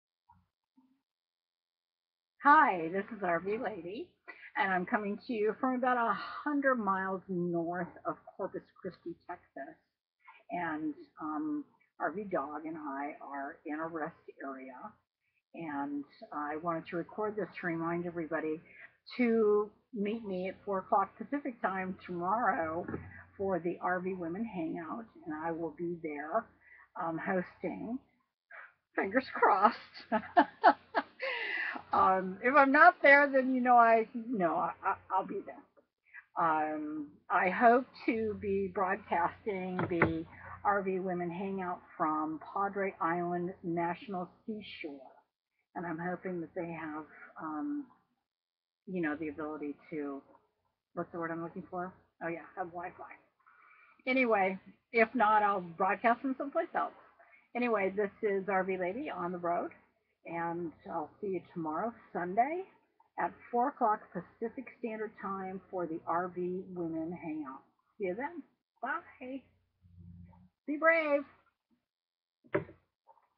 RV Women Hangout REMINDER - Sunday @ 4 PM Pacific Standard Time
I hope to broadcast from Padre Island National Seashore! What could go wrong? Hope to see you all tomorrow at 4 PM Pacific Standard Time! :) BE BRAVE!